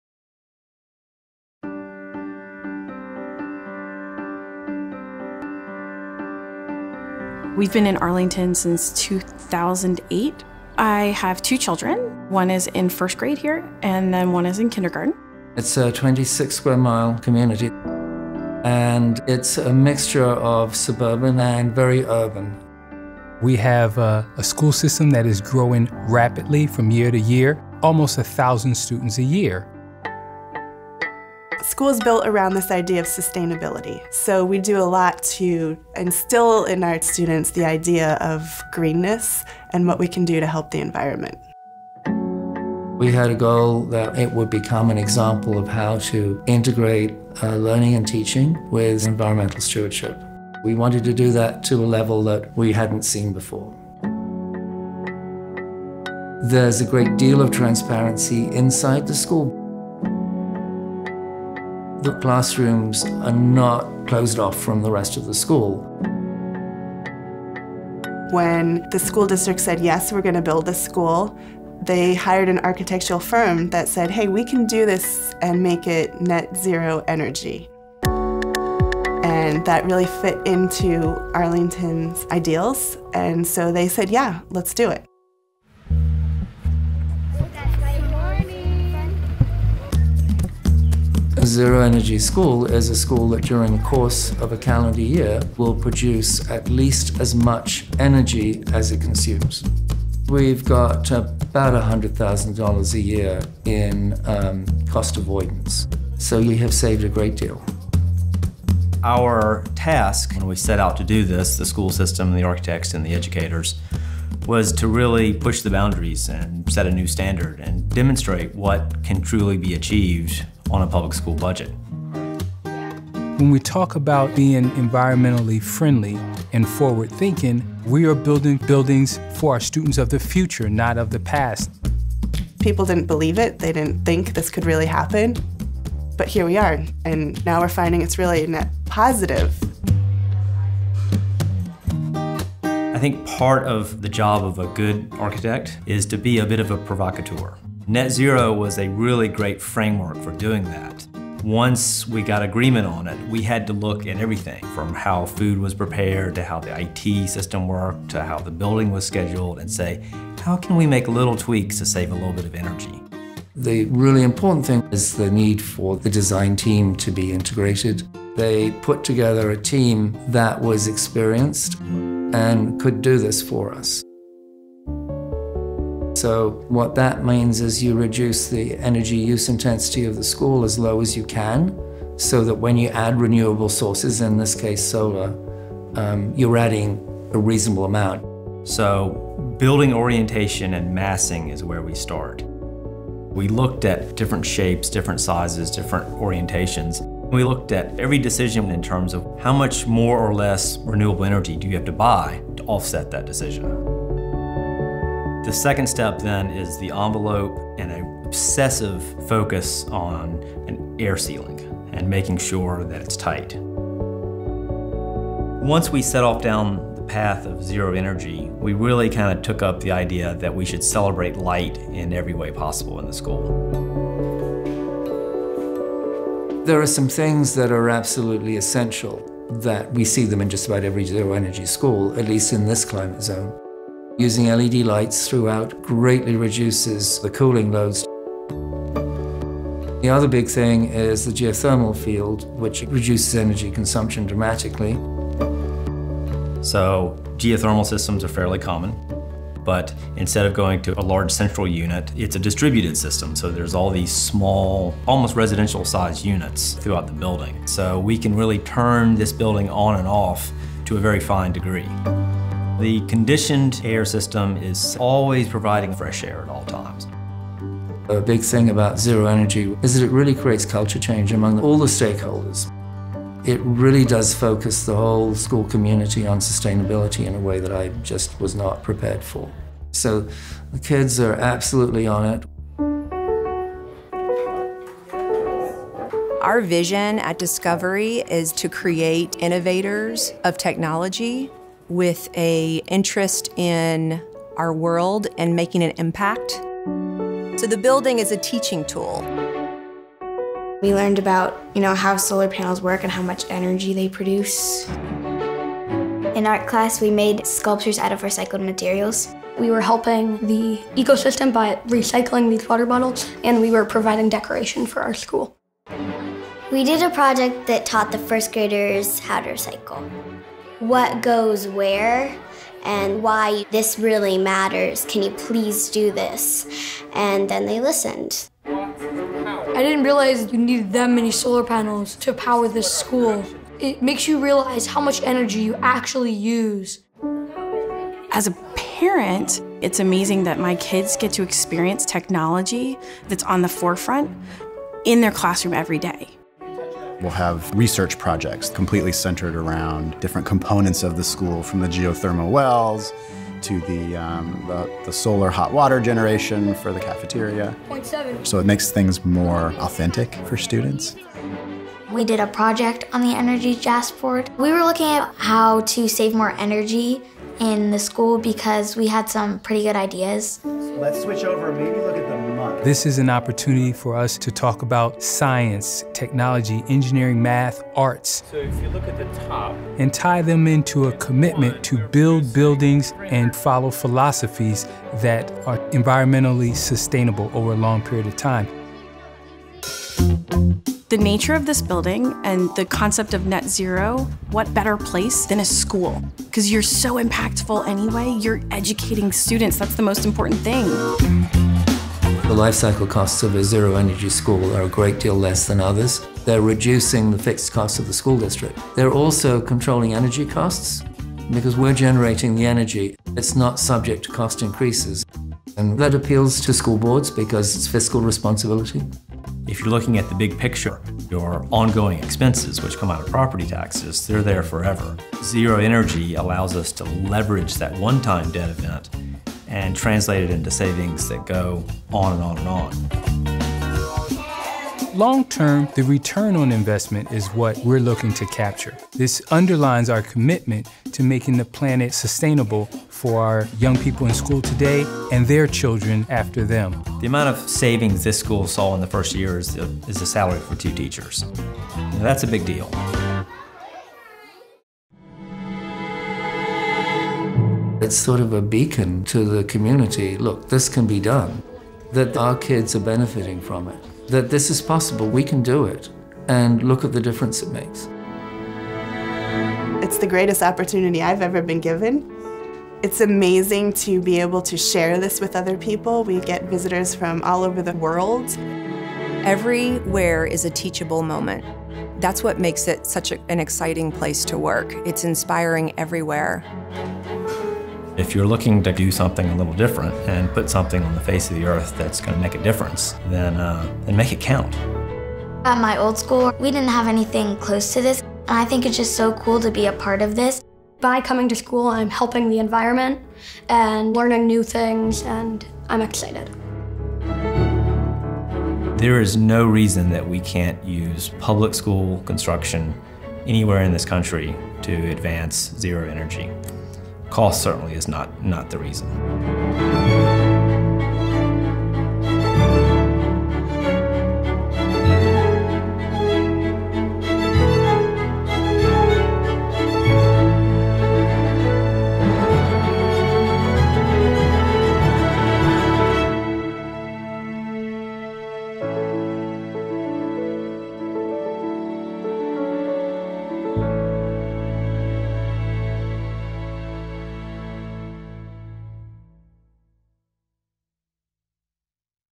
Building Brighter Futures Through Zero Energy: Discovery Elementary School
Currently experiencing a population boom, Arlington County is facing massive growth in the next decade and is seeking to add half a million square feet in educational facilities. During competitive design procurement for a new elementary school, VMDO suggested a zero energy goal could be accomplished within the given budget. Proponents at the district level who had been championing energy efficiency were receptive because sustainability was a core value of the project from the start, but they were skeptical that it could be done within the budget aimed at LEED Silver. Not only did the project end up coming in under budget, including the solar array, but the building is also more efficient than originally predicted. Now Discovery saves $100,000 per year in utility costs, enough to cover the salaries of two teachers. Thanks to the U.S. Department of Energy for and the National Renewable Energy Laboratory for making this video possible and for their commitment to zero energy schools.